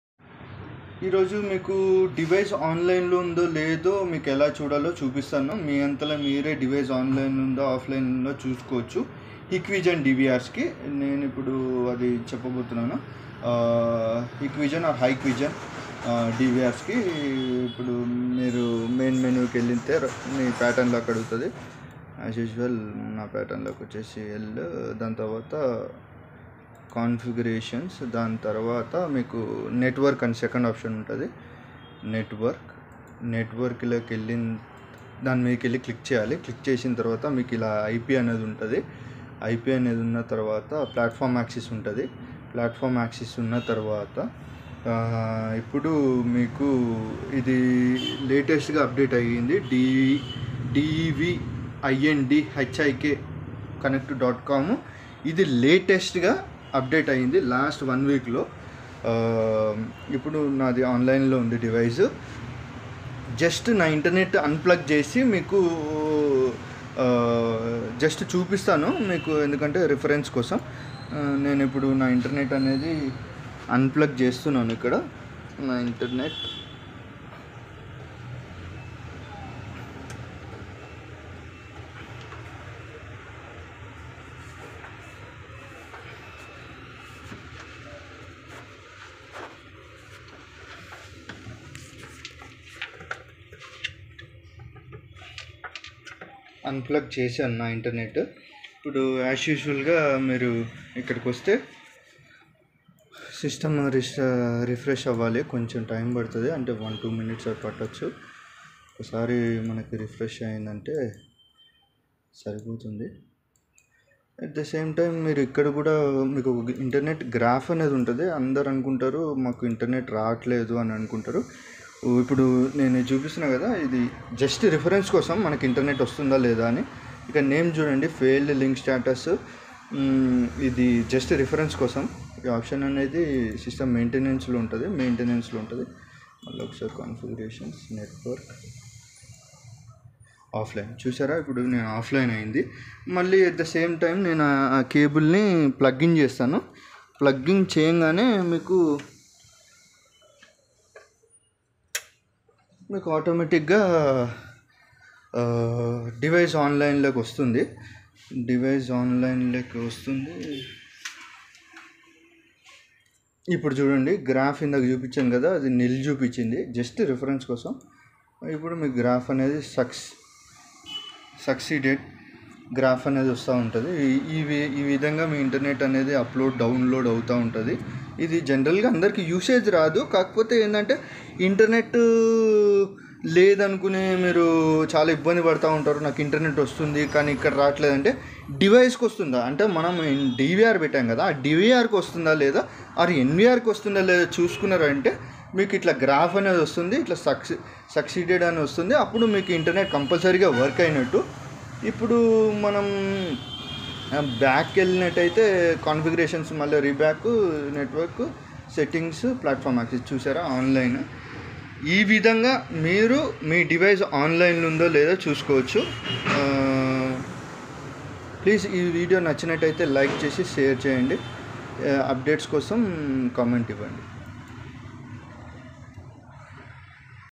Telugu How to check dvr/nvr is in offline or online
How to check DVR/ NVR status  is in offline or online for  Hikvision dvr   in telugu